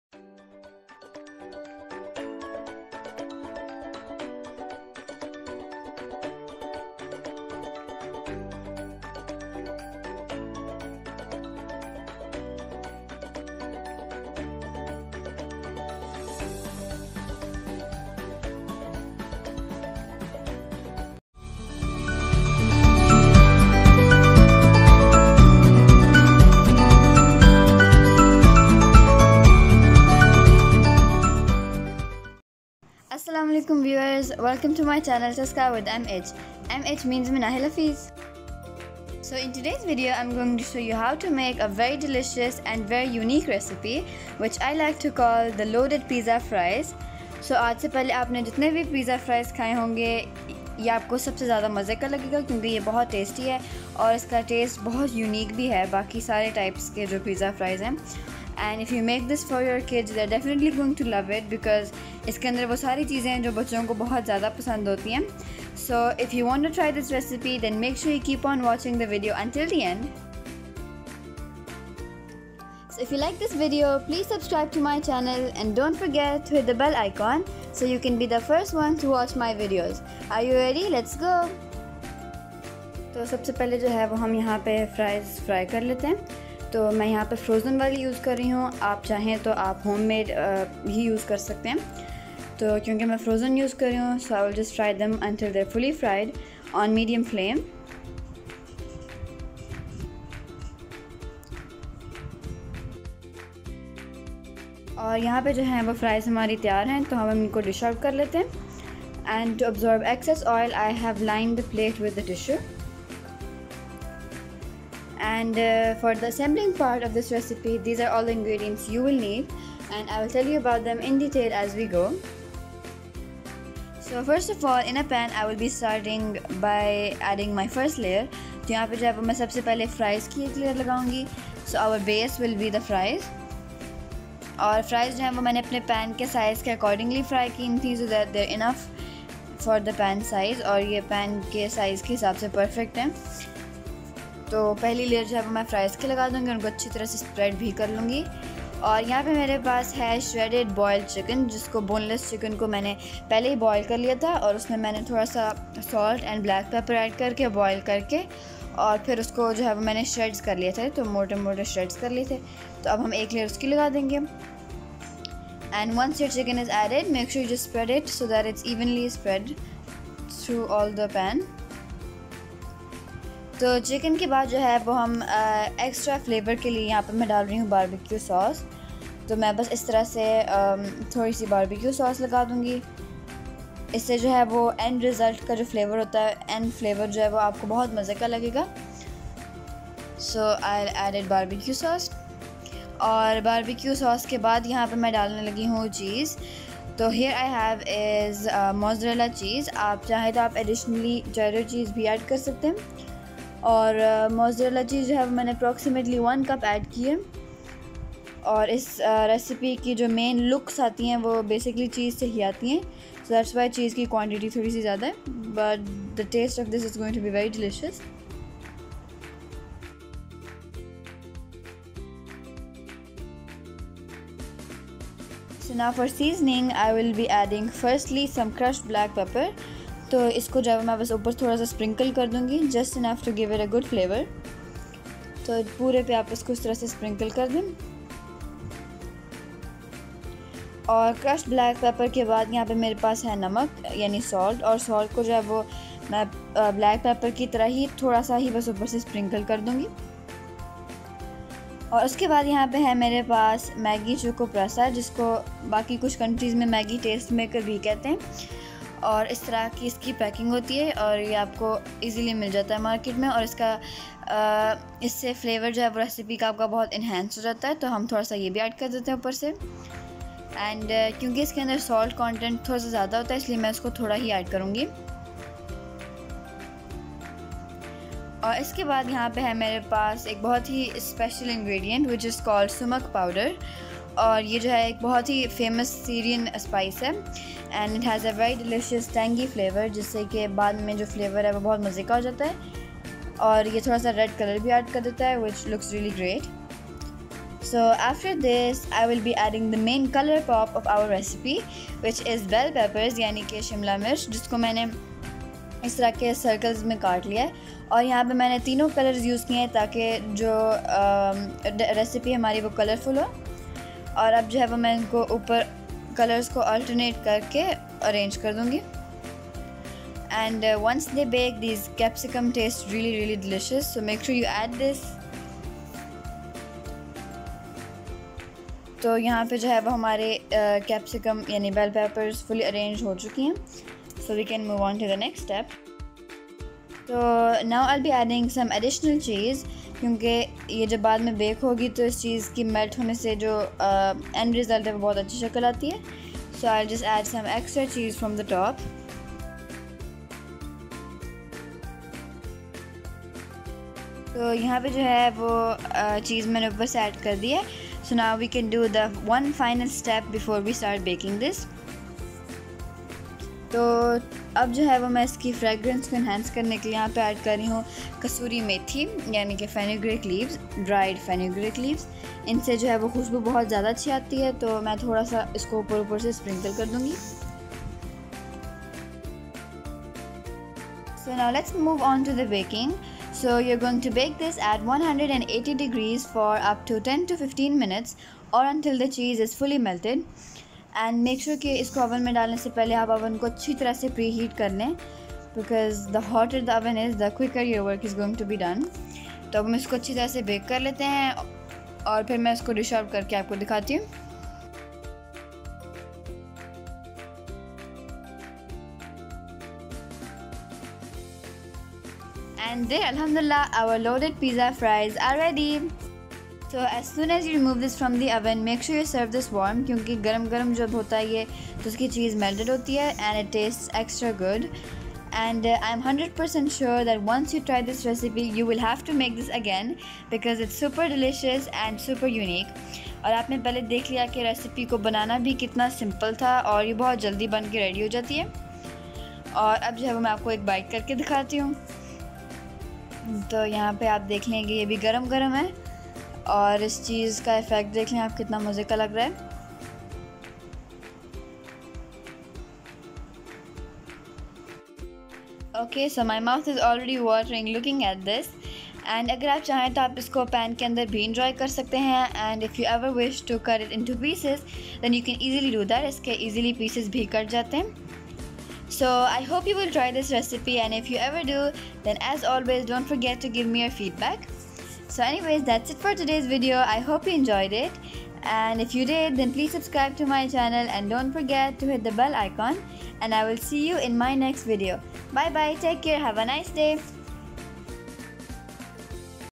Loaded pizza Fries recipe (pizza fried for cheese lovers) BY MH
ingredients:
-fry fries either use homemade or frozen
- boneless boiled chicken
- BBQ Sauce
- 1 cup  mozzarella cheese
-pinch of salt and pepper
-Maggi Cukup Rasa
-Syrian sumac powder
- bell pepper slices of all 3 colors
-extra cheese

* bake at 180 degree Celsius for 10-15 mins